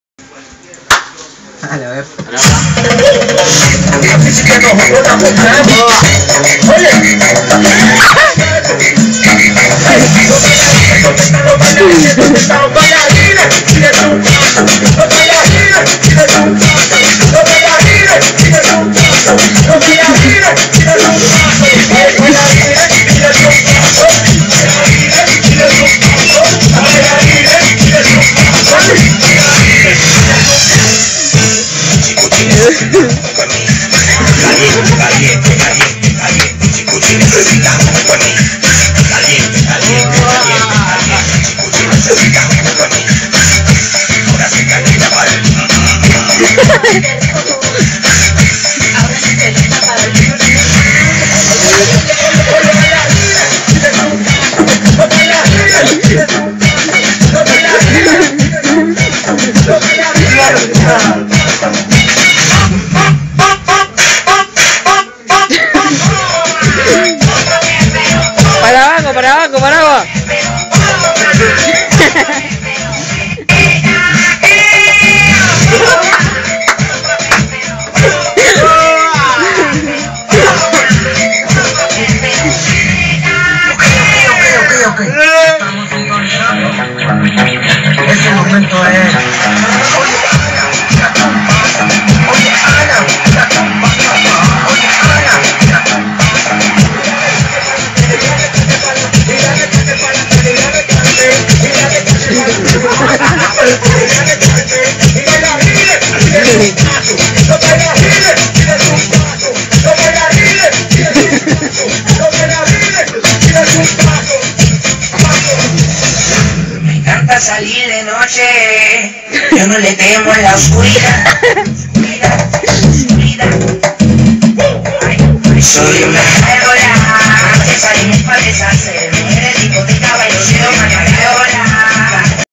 "El Paragua" bailando unos remix ♫
El "PARAGUA" es un chico cejon de origenes paraguayos que vino a nuestro pais en busca de emociones y aventuras.